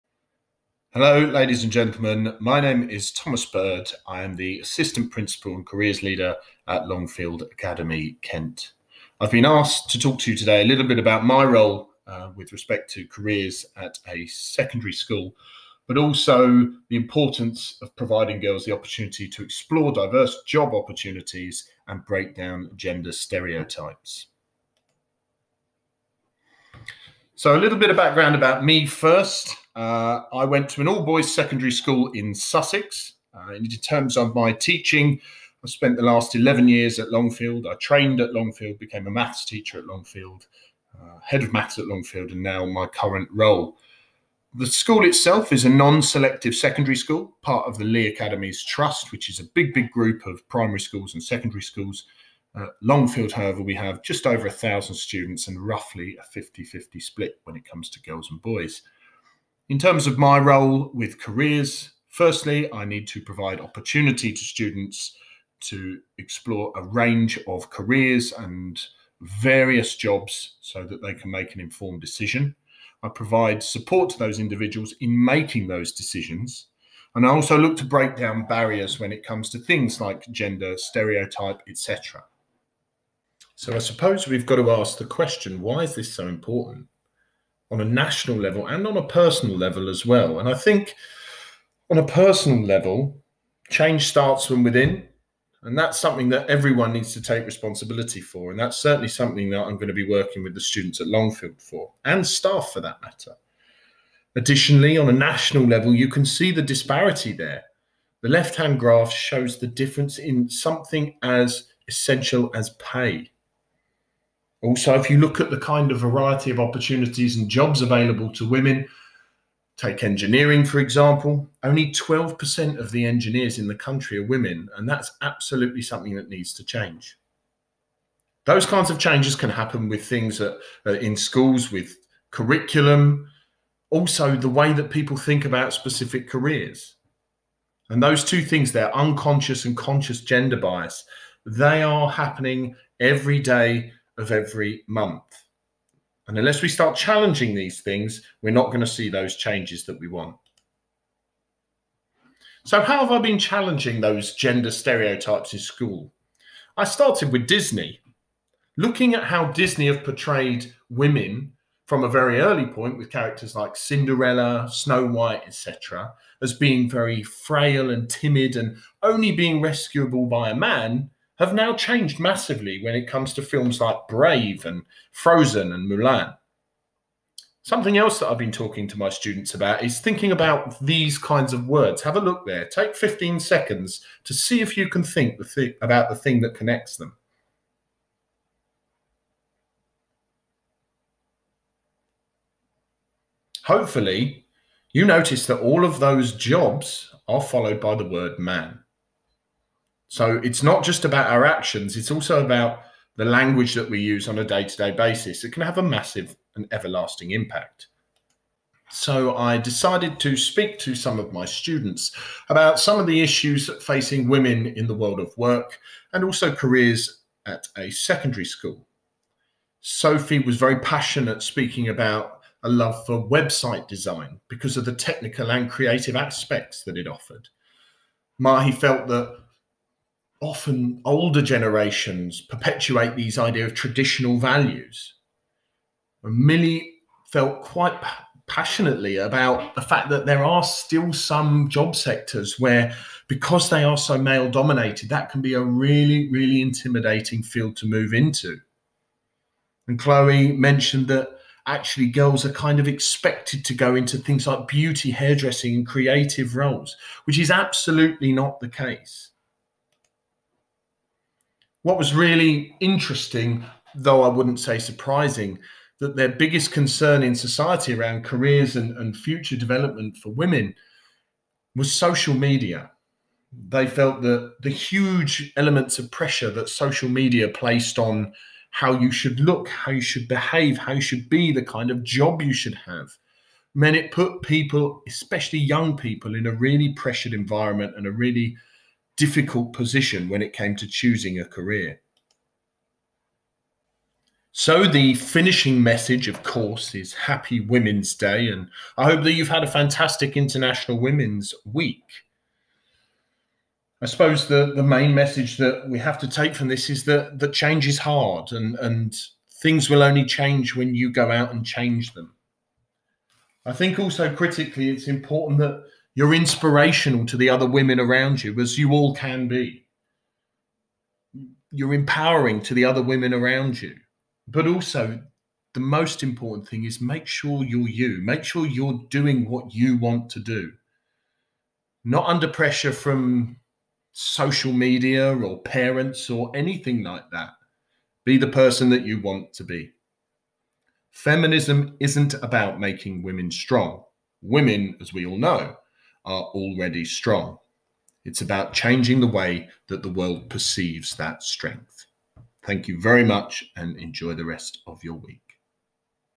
Thomas Bird - Assistant Principal and Careers Leader at Longfield Academy, Kent
As part of International Women's week 2021, Thomas Bird, Assistant Principal and Careers Leader at Longfield Academy talks about how he supports girls and all young people with regards to careers and why it is so important!